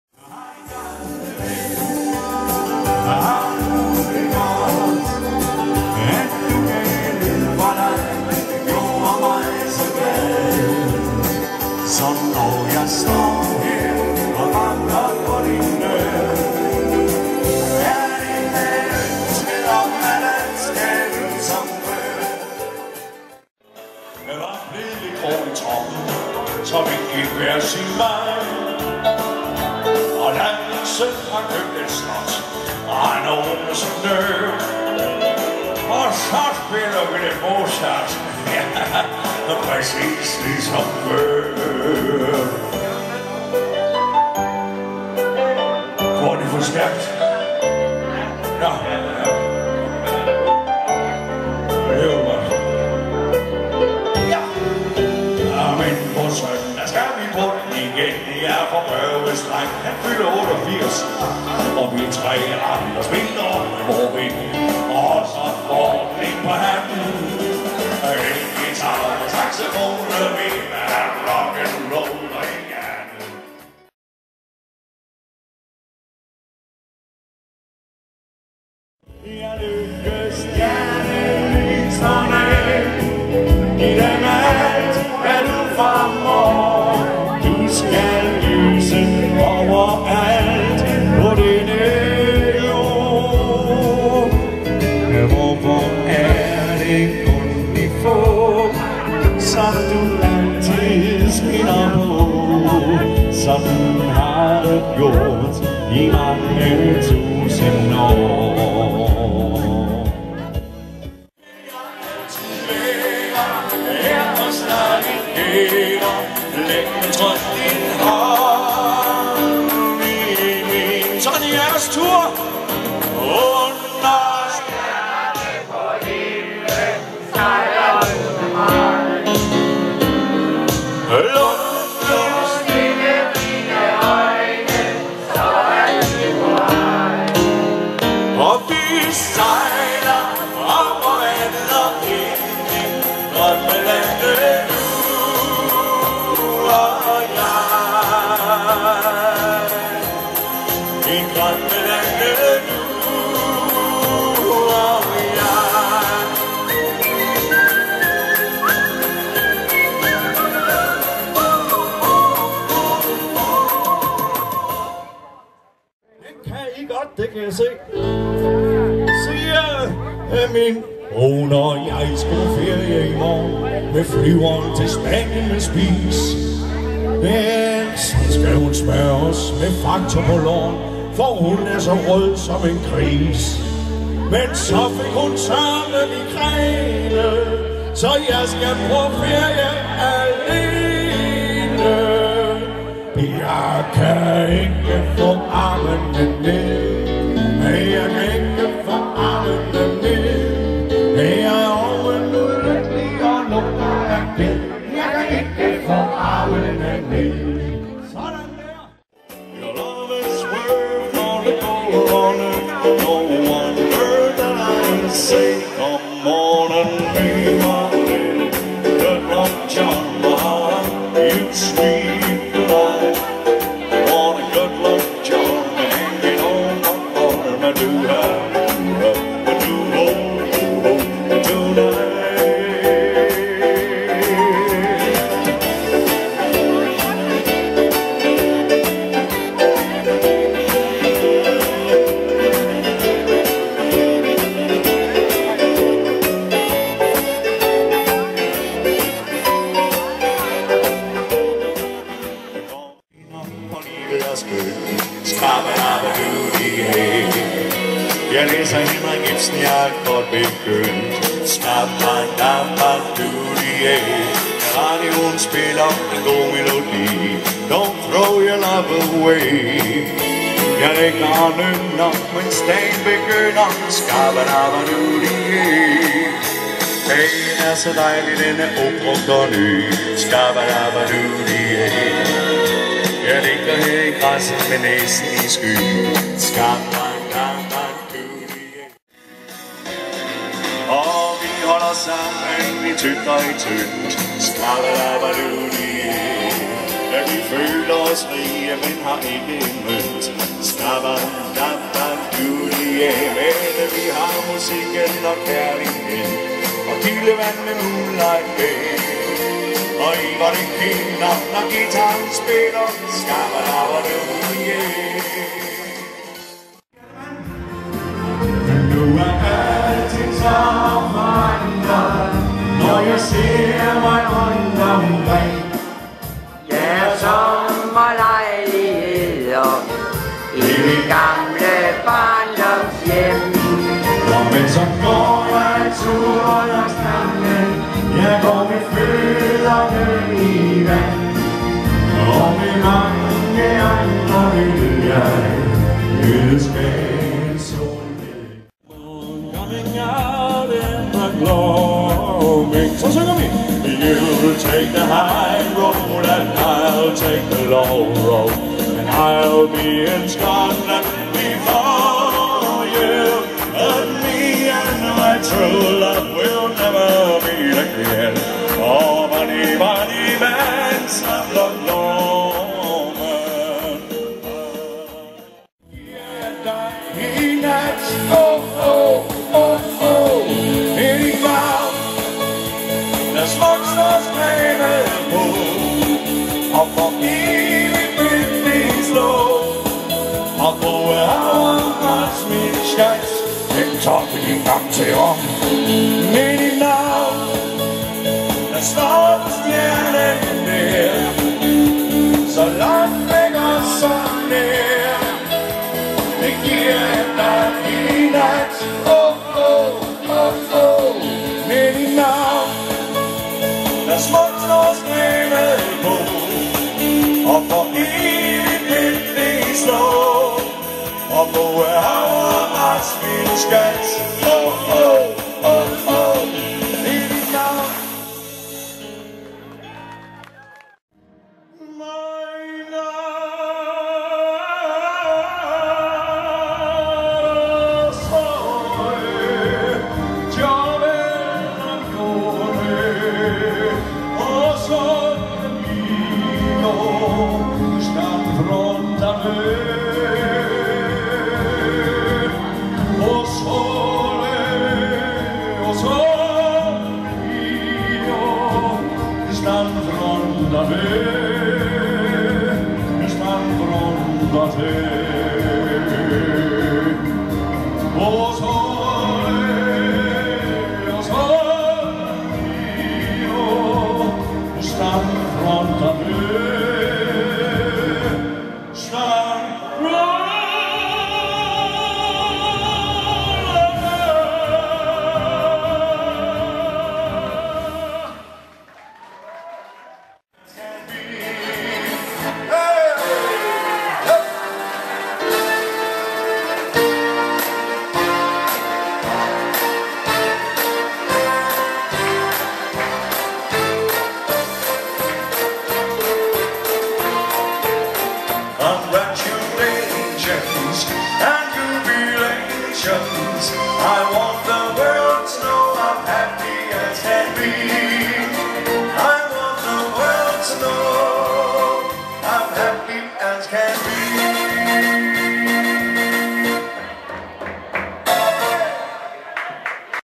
TILLYKKE til ROBIN
Klip fra mange forskellige sange fra Robin- der mestrer alle "stil" arter --- i anledning af Robins fødselsdag.
Film : Hanne Melin